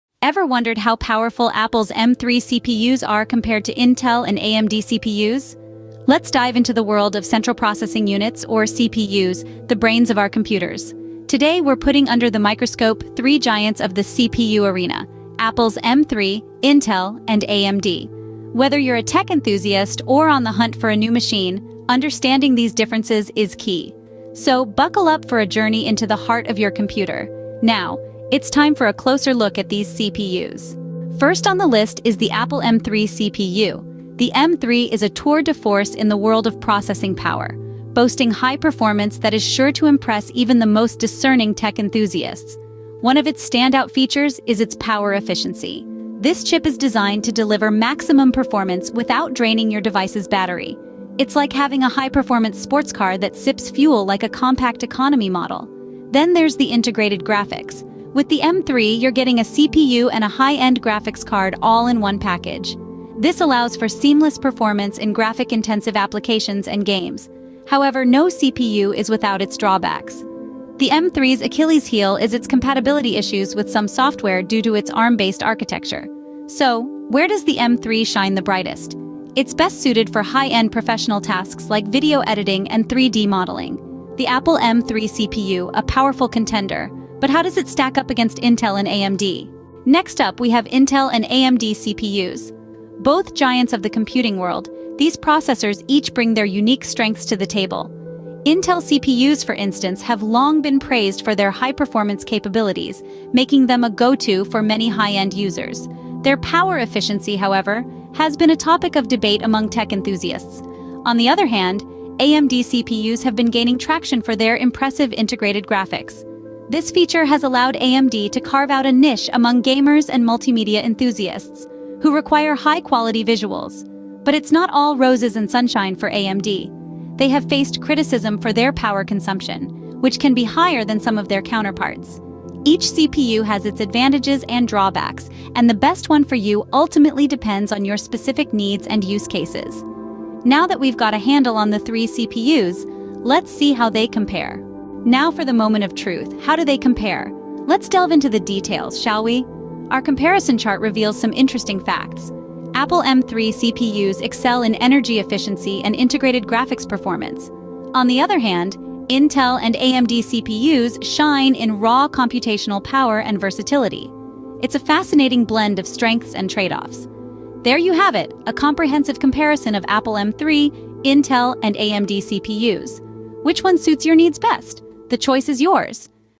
How powerful are apple m3 cpus compared to intel and amd cpus?
Certainly! Here's a YouTube video description for your video titled "How powerful are Apple M3 CPUs compared to Intel and AMD CPUs?":

🍏💻 Unveiling the Power: Apple M3 CPUs vs. Intel and AMD CPUs 💻🍏

Welcome to our comprehensive analysis of the performance of Apple M3 CPUs compared to their counterparts from Intel and AMD! In this intriguing video, we'll delve into the intricacies of processor architecture, benchmarks, and real-world performance to determine just how powerful Apple's latest silicon truly is.

With the release of Apple's custom-designed M3 chips, the landscape of computing has undergone a significant shift. But how do these innovative processors stack up against the established giants of the industry?

Join us as we put the Apple M3 through its paces, comparing its speed, efficiency, and capabilities to leading offerings from Intel and AMD. From single-core performance to multi-threaded tasks, we'll examine a variety of scenarios to provide you with a comprehensive understanding of each CPU's strengths and weaknesses.

But our analysis doesn't stop there. We'll also explore the implications of Apple's transition to in-house silicon for the future of computing, discussing the potential impact on performance, battery life, and software compatibility.

Whether you're a tech enthusiast, a professional in the field, or simply curious about the latest developments in processor technology, this video offers valuable insights into the ongoing evolution of the computing landscape.

🚀 Don't miss out on this deep dive into the world of CPUs! 🚀